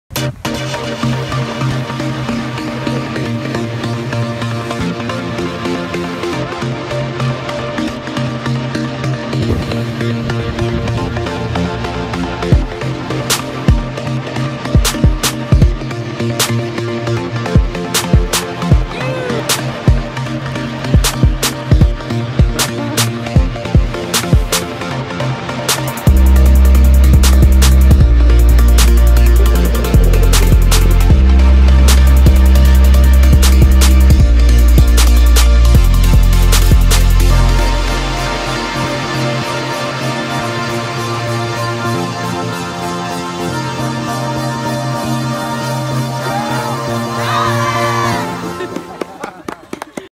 College Ski Trip 2020 | Cronton Sixth Form College | La Plagne, France
Some great video clips from the Cronton Sixth Form 2020 Ski Trip to La Plagne, France!